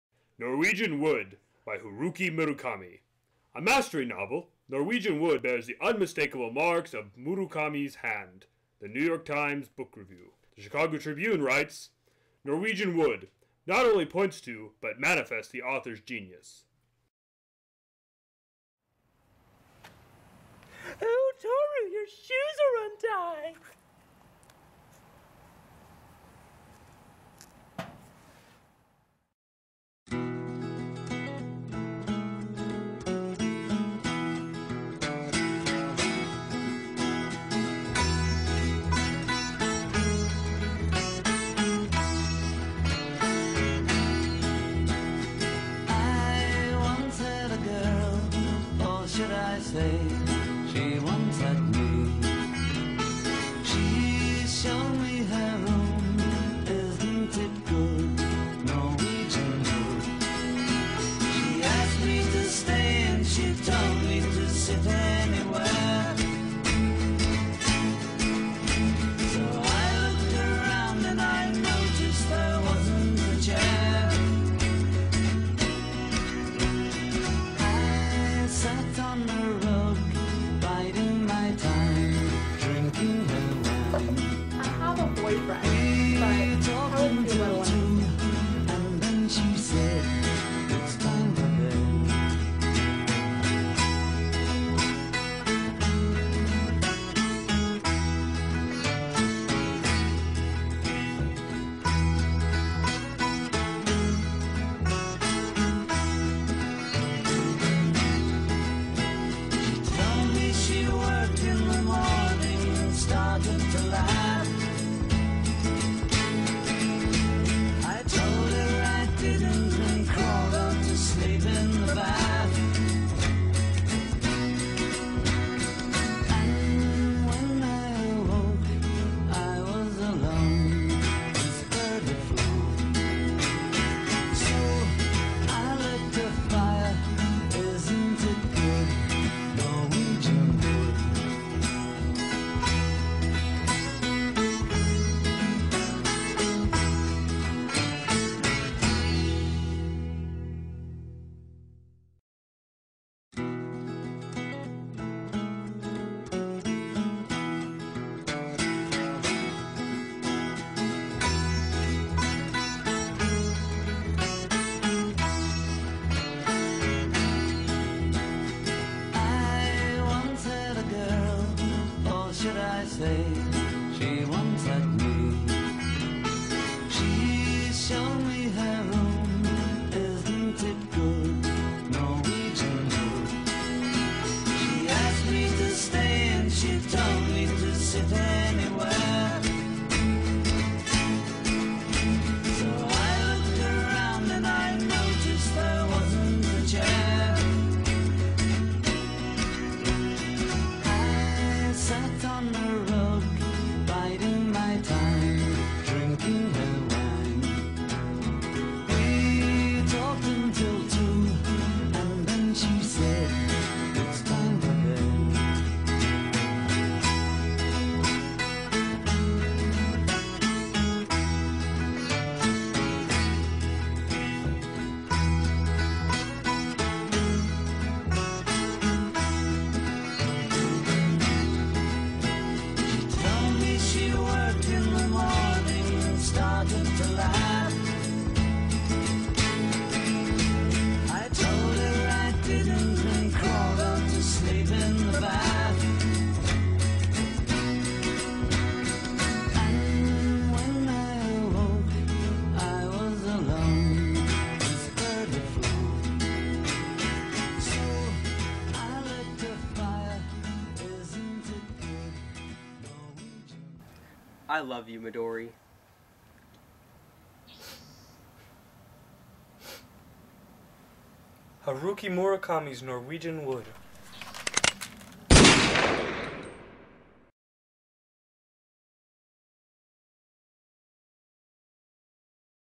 NORWEGIAN WOOD TRAILER
A trailer for Haruki Murakami's novel Norwegian Wood.